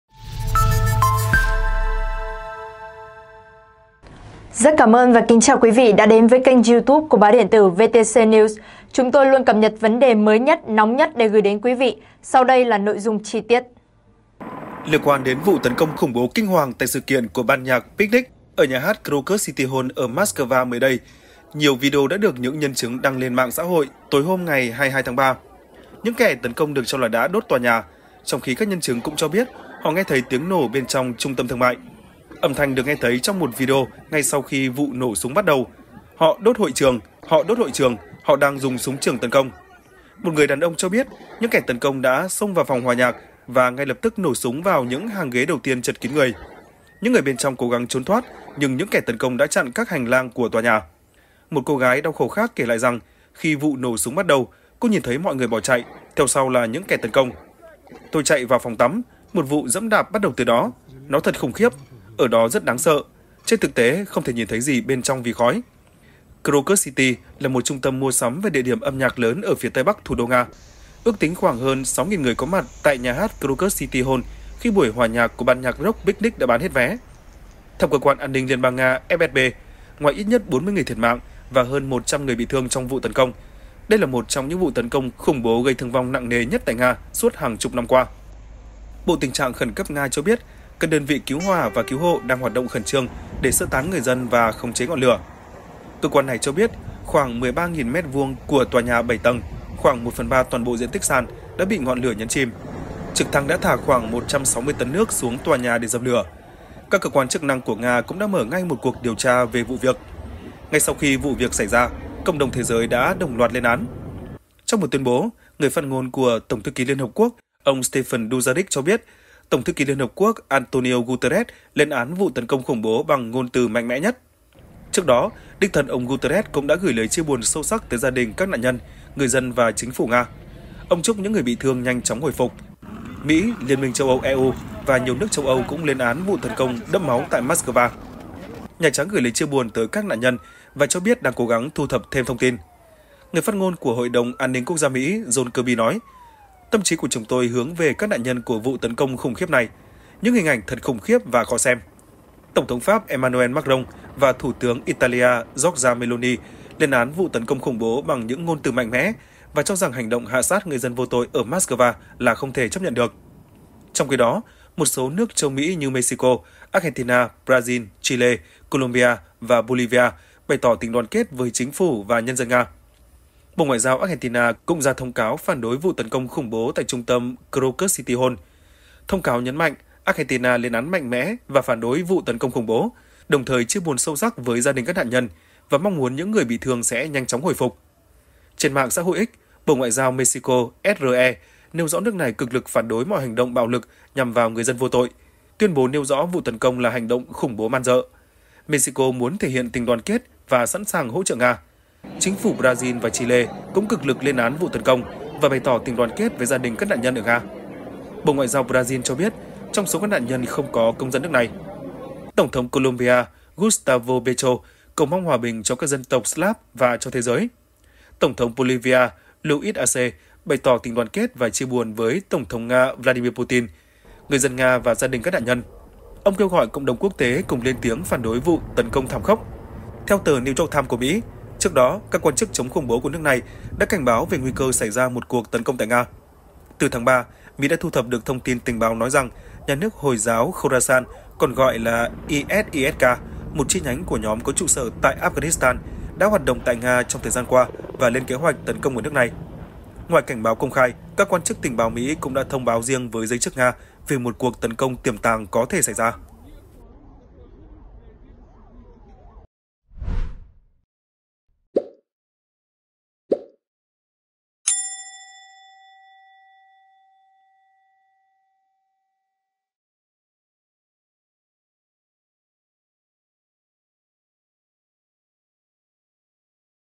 Khủng bố kinh hoàng ở Moscow: Nhân chứng hoảng loạn kể lại khoảnh khắc các tay súng ập vào
Nga Ukraine mới nhất | Khủng bố kinh hoàng ở Moscow: Nhân chứng hoảng loạn kể lại khoảnh khắc các tay súng ập vào
Liên quan đến vụ tấn công khủng bố kinh hoàng tại sự kiện của ban nhạc Picnic ở nhà hát Crocus City Hall ở Moskva mới đây, nhiều video đã được những nhân chứng đăng lên mạng xã hội, tối hôm 22/3.